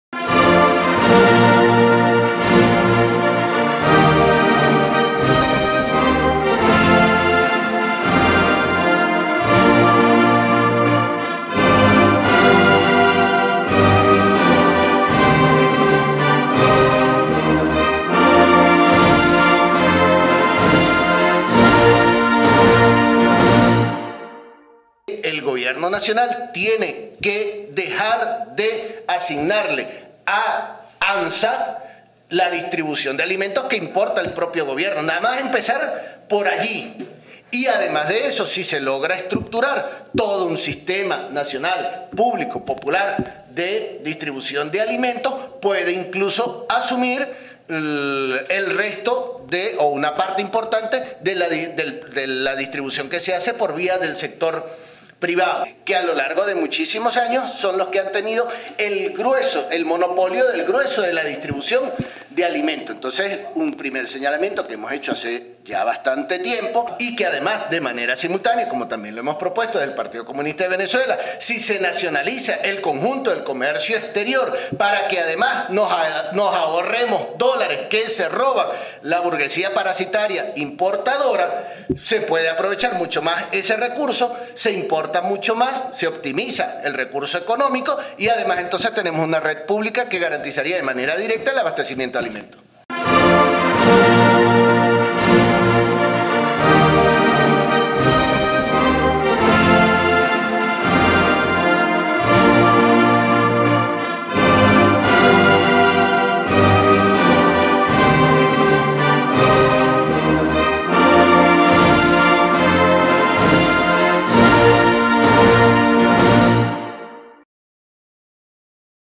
PCV: Es posible enfrentar el desabastecimiento con un Sistema Nacional Público y Popular
Caracas, 15 mar. 2016, Tribuna Popular TP.- El Partido Comunista de Venezuela (PCV), reiteró al gobierno nacional la necesidad de crear un Sistema Nacional Público y Popular de Distribución que asegure el abastecimiento, principalemte de alimentos a la población y debe dejar de entregarle al sector privado esos artículos porque siguen especulando con ellos.

Además, la tolda del Gallo Rojo volvió a cuestionar la entrega de dólares preferenciales al sector privado y exigio la nacionalización total de sistema de importaciones y exportaciones del país.

Así lo dio a conocer, Carlos Aquino en rueda de prensa del pasado lunes 14 de marzo.